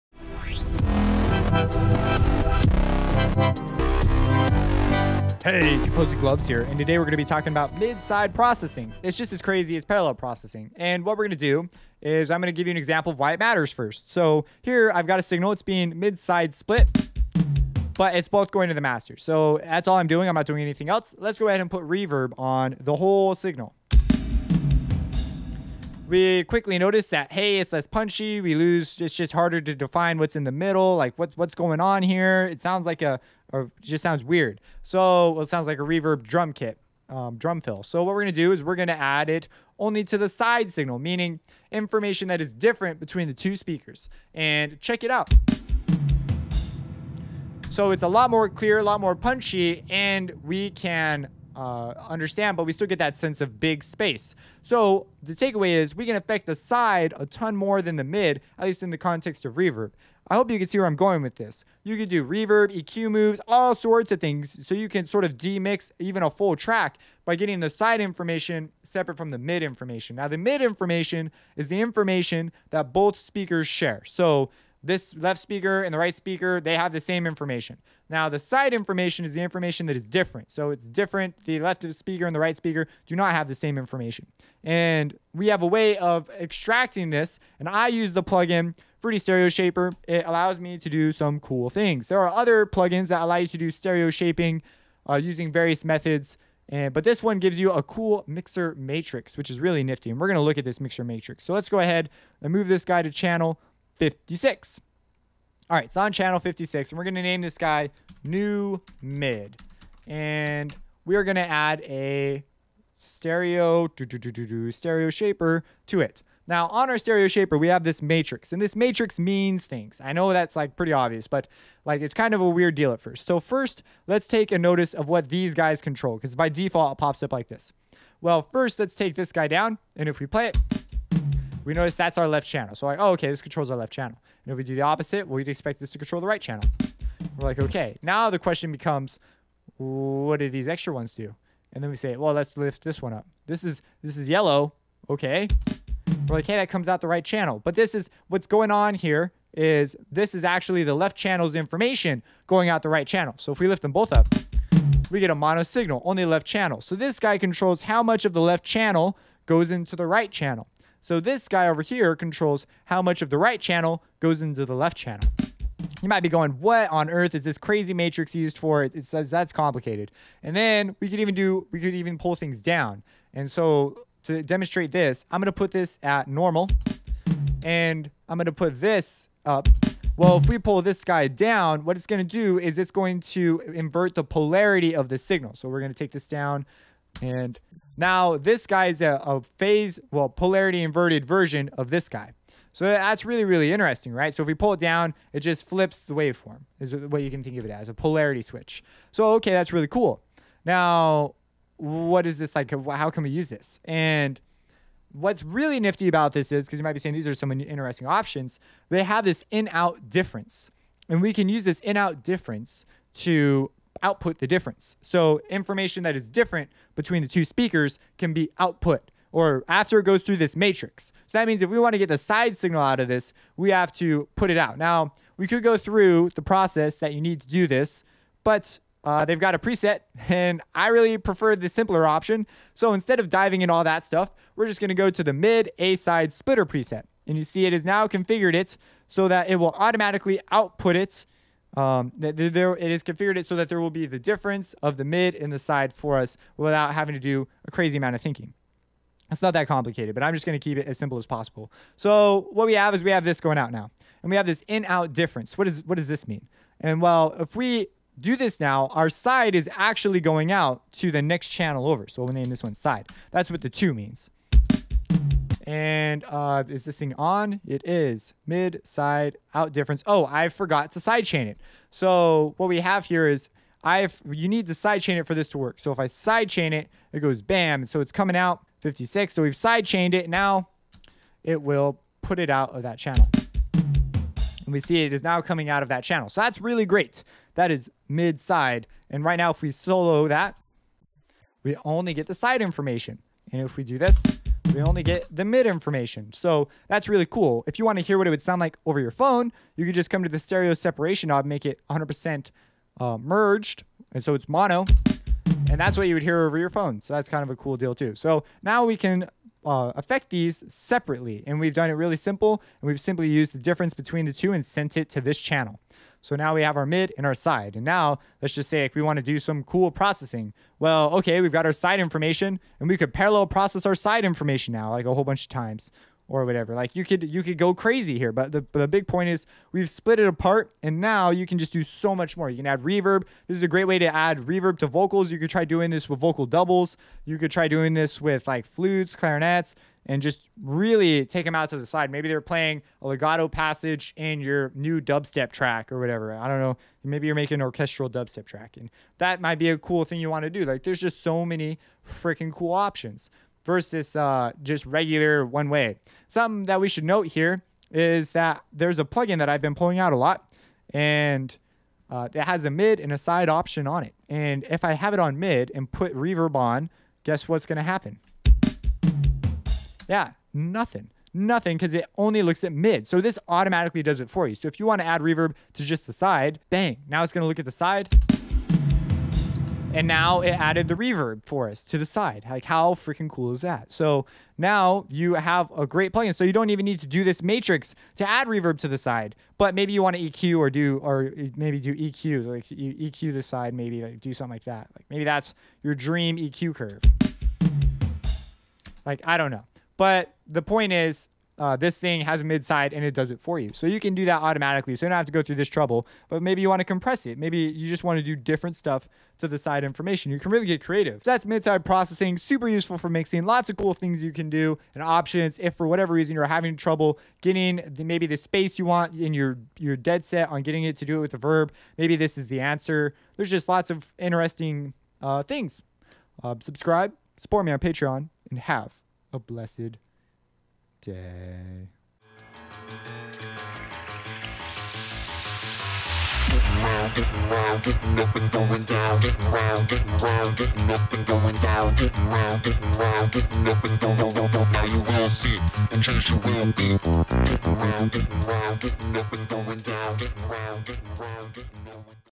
Principles of Mixing 40 - Mid/Side Processing
Mid/Side processing is sorta like parallel processing but in a stereo kind of way. Its super powerful and useful tool for creating new sense of space and FX while maintaining a part of the original sound in an image.